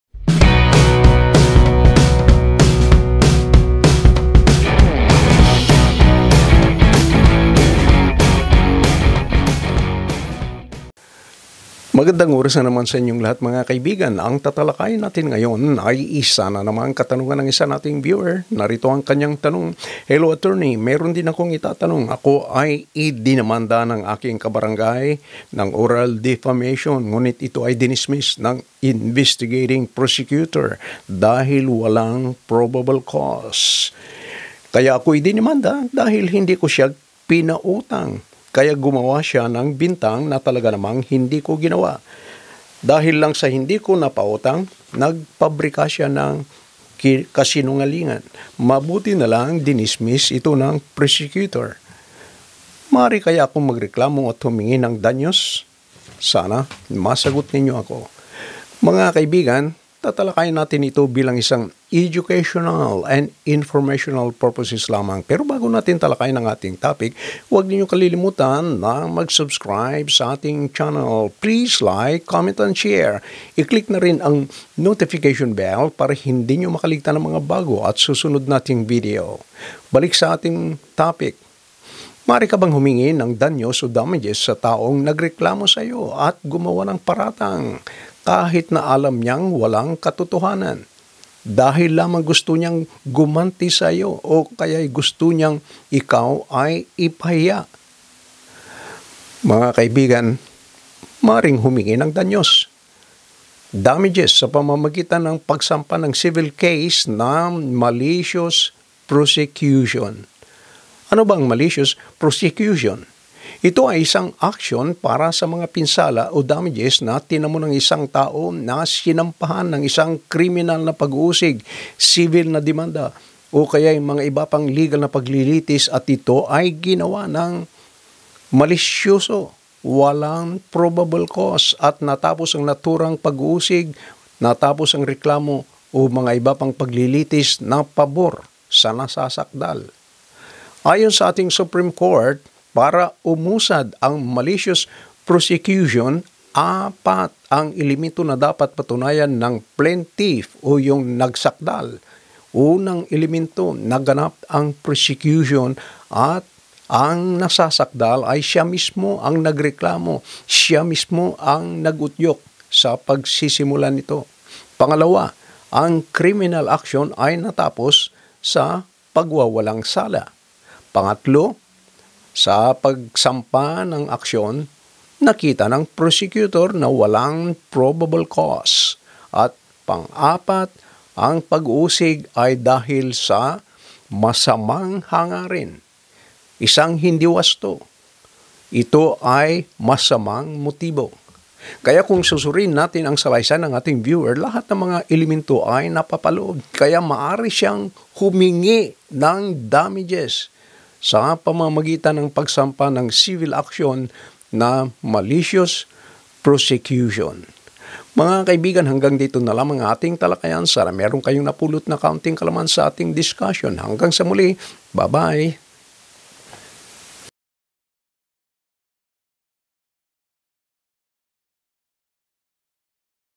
Pwede bang sampahan ng danyos ang nagreklamo ng walang katotohanan dahil ito ay gawagawa lamag?
Maaring sampahan ng malicious prosecution ang taong malisyosong nagreklamo sa iyo ng walang katotohanan. Ang malicious prosecution ay  isang aksyon para sa mga pinsala o damages na tinamo ng isang tao na sinampahan ng  isang kriminal na pag uusig, sibil na demanda, o iba pang mga legal na paglilitis at ito ay ginawa nang malisyoso at walang probable cause at natapos ang naturang pag uusig, suit, o iba pang mga paglilitis sa pabor ng nasasakdal.